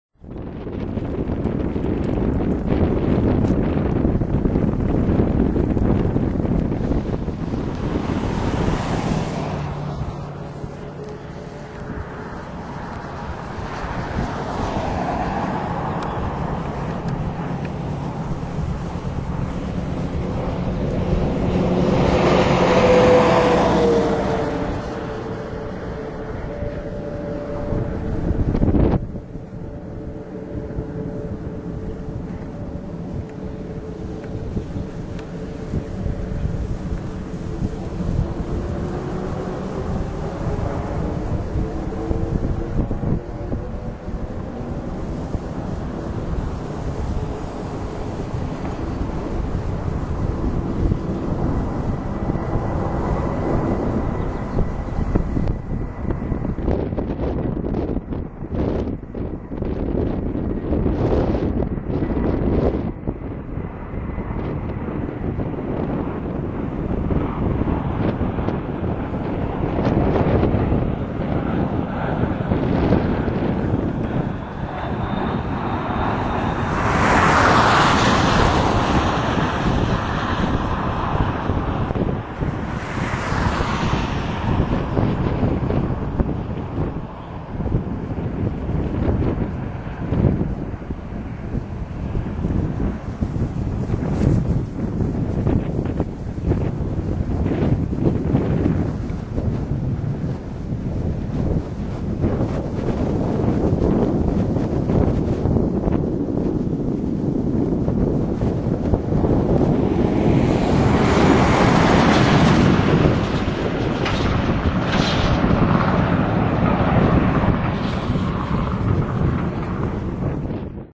KC-10 recovery
A KC-10 returns to Nellis AFB after an exercise at the near by MOA's. Call Sign GULF.